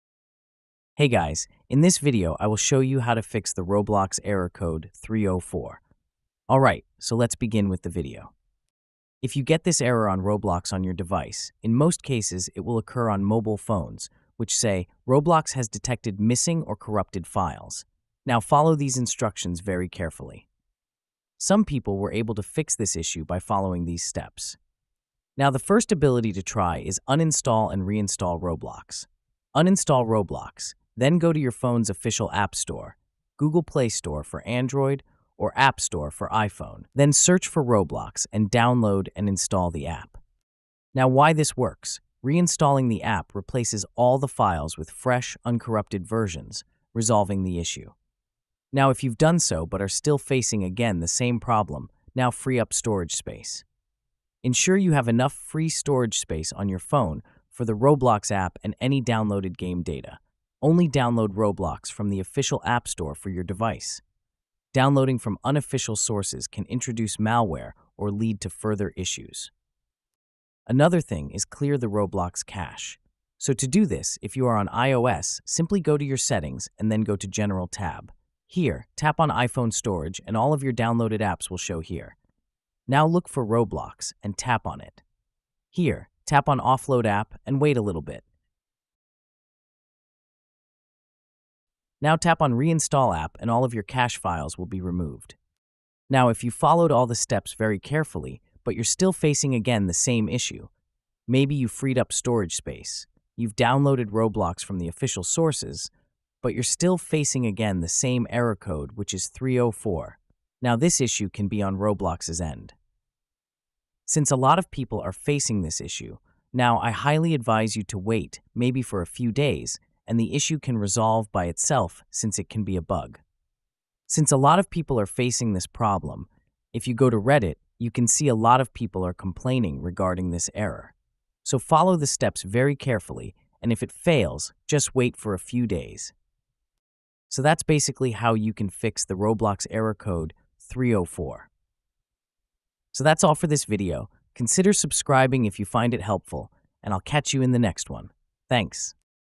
Roblox Error Code 304 (Quick Fix)
Getting Roblox Error Code 304? This quick fix tutorial will help you solve it in just a few steps! Error 304 usually means there’s a problem with your request being blocked or redirected—often due to outdated browser data or connection issues. In this video, I’ll show you how to clear cache, reset your browser or app, and fix Error 304 on both PC and mobile.